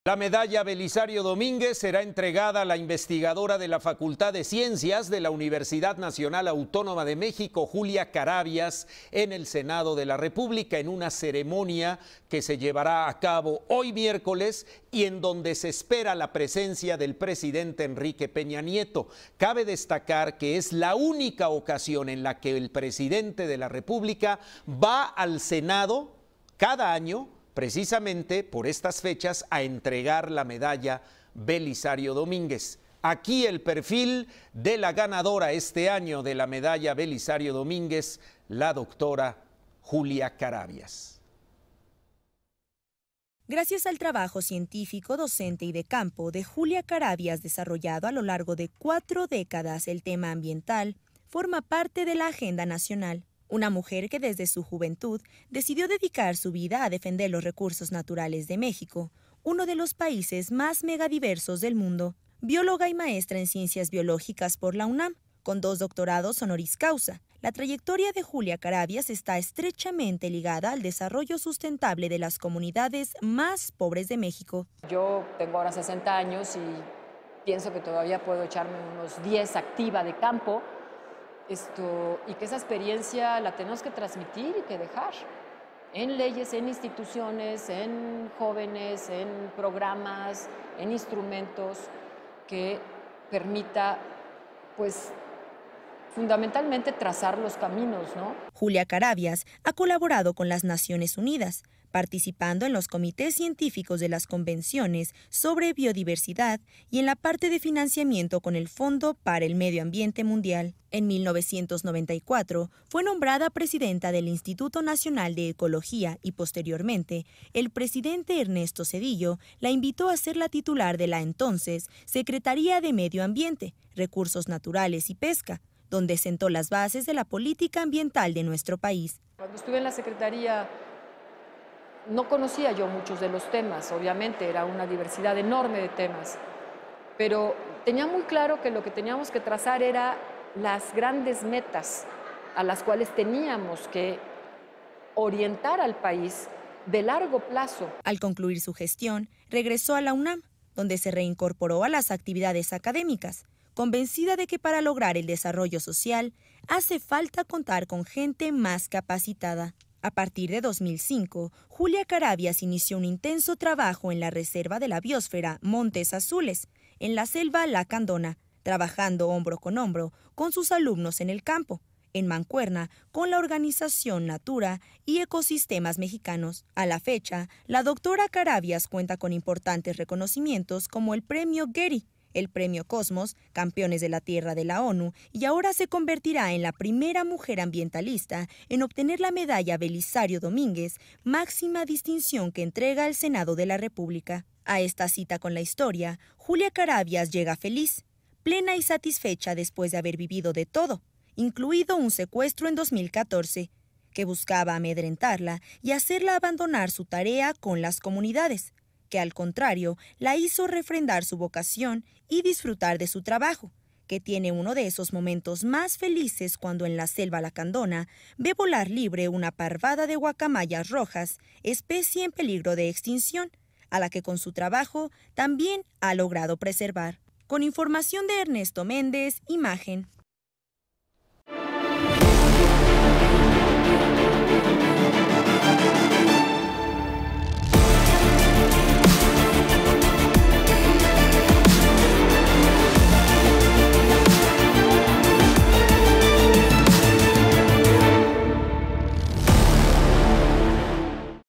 ¿Quién es Julia Carabias, ganadora de la Belisario Domínguez 2017?
El pleno del Senado de la República ratificó por mayoría entregar la Medalla Belisario Domínguez en su edición 2017 a Julia Carabias Lillo, bióloga y ambientalista de la Universidad Nacional Autónoma de México (UNAM).    06 diciembre de 2017

Conductor: Martín Espinosa.
Horario: Lunes a Viernes @07:00-09:30.